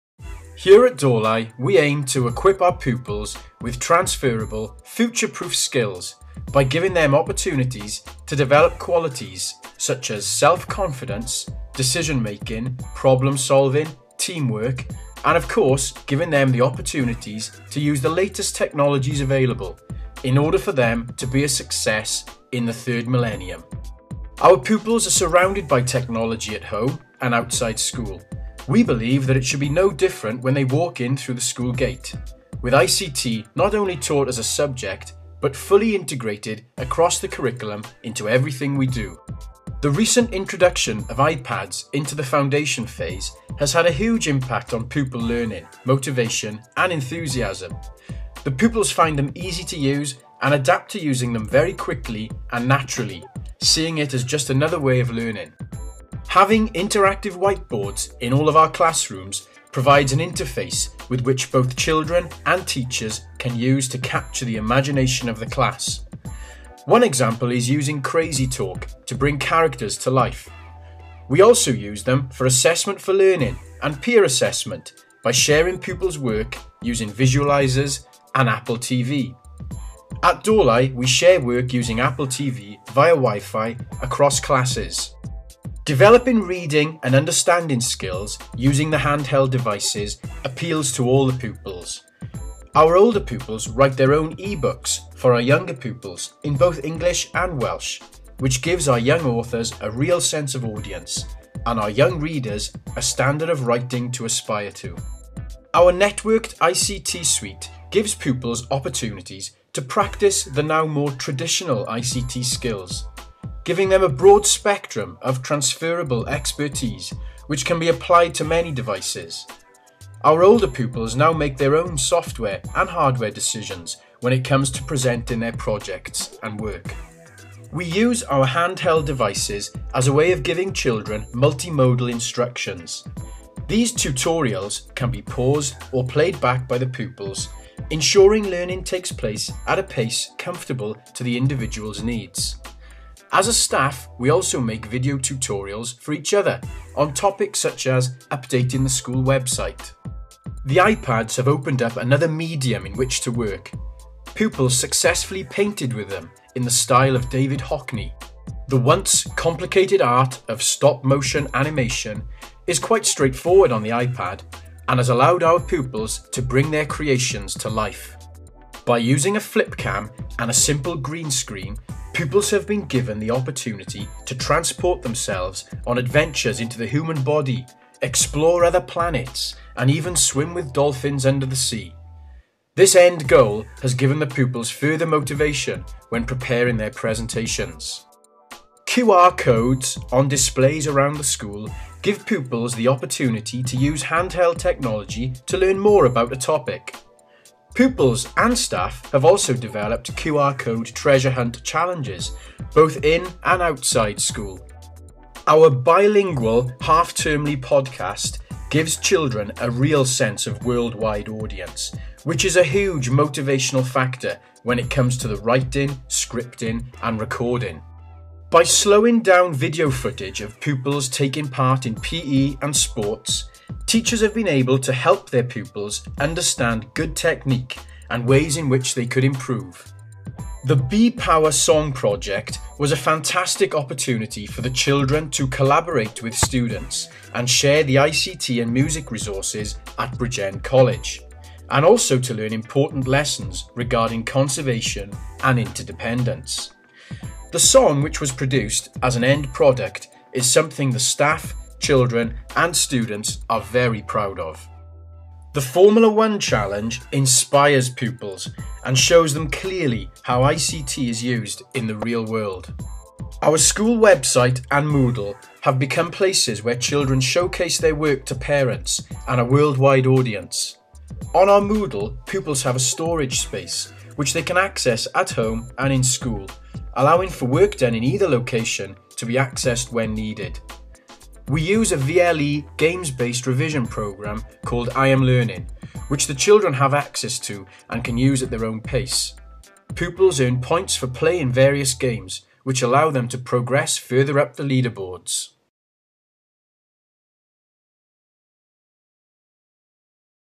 Ysgol Gynradd Dolau Primary School: 3rd Millennium Learning Award video
The video here is a promotional video submission for the 3rd Millennium Learning Award. It is freely available under CC licence for other schools and educators to learn from.

This video shows ways that the school has incorporated new ways of learning enabled by technology into their everyday work. Further details, together with information about joining the scheme joining the scheme can be found Naace has developed the 3rd Millennium Learning Award to showcase best practice.The focus of the Award is learning, not technology.

The 3rd Millennium Learning Award submissions are in two parts. The first is a promotional video for the school community and the second is a commentary designed for educators to highlight the ways that technology is supporting new ways of learning.